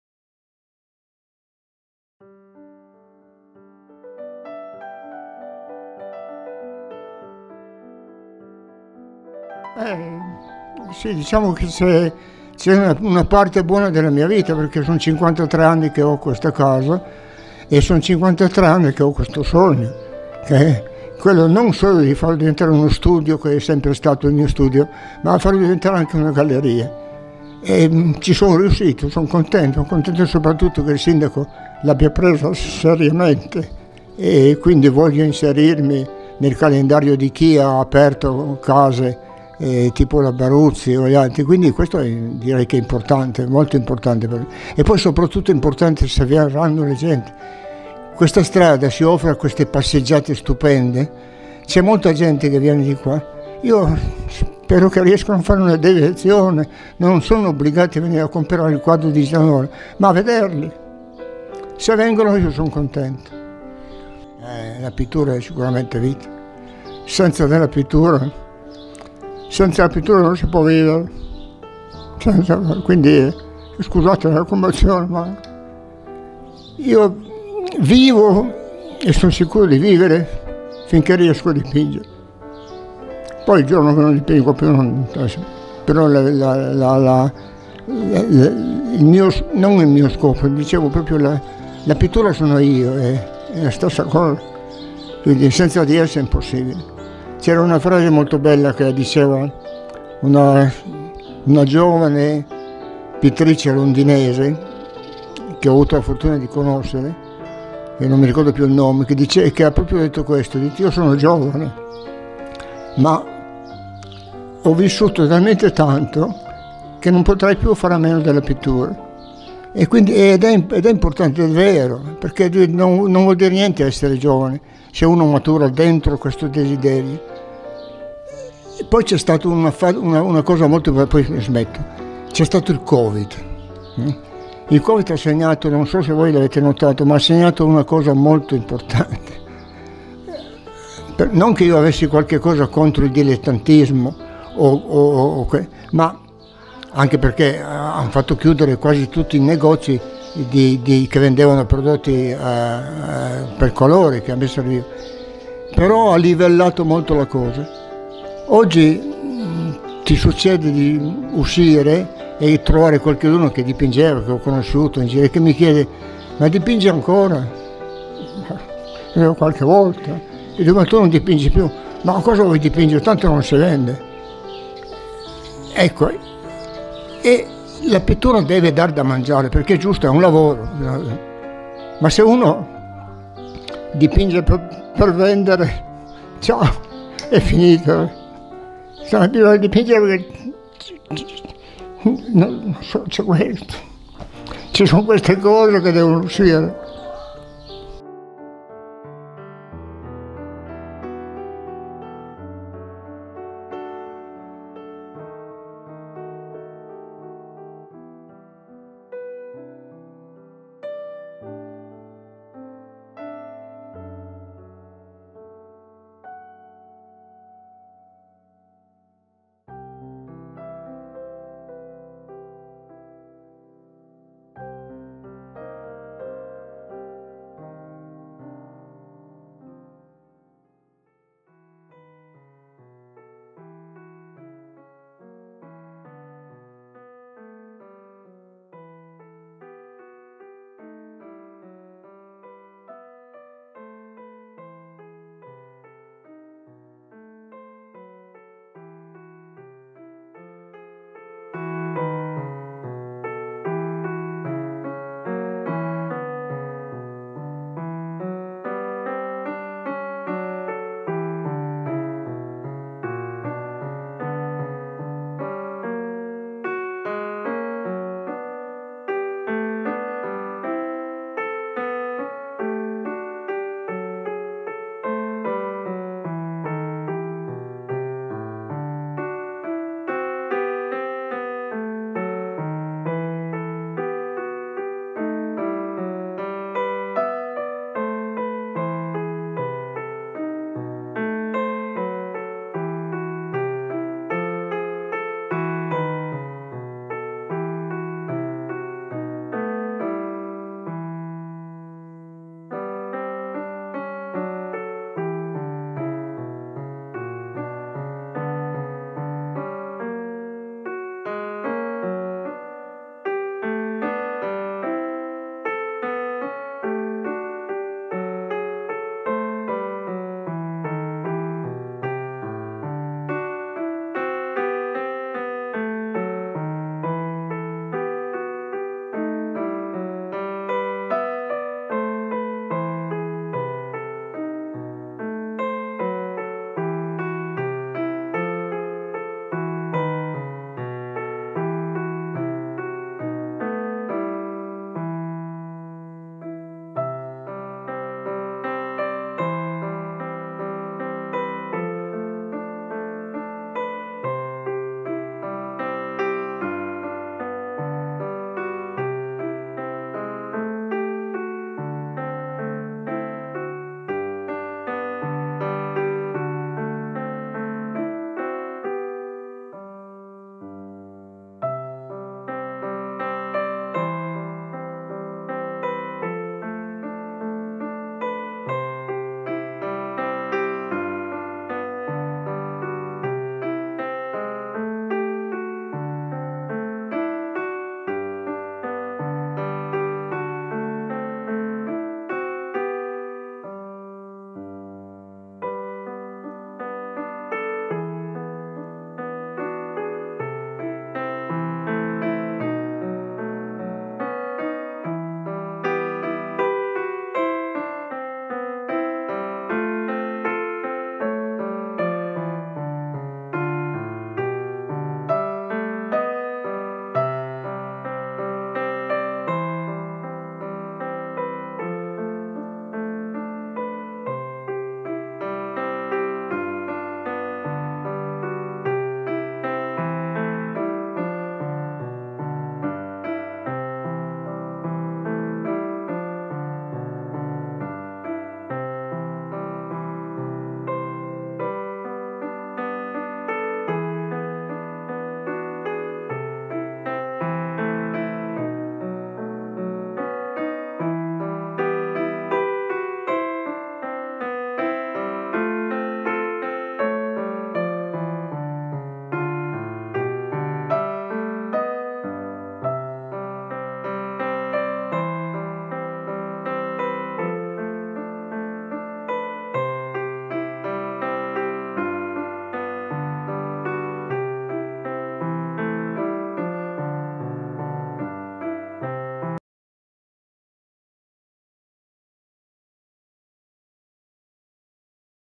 CASTELLAMONTE - .... Ciao Miro... vogliamo ricordarti così
Miro Gianola, mancato ieri pomeriggio. Pittore e scultore, nasce proprio a Castellamonte nel 1939 e la sua carriera inizia da giovanissimo. Infatti, realizza il suo primo dipinto quando aveva appena 12 anni.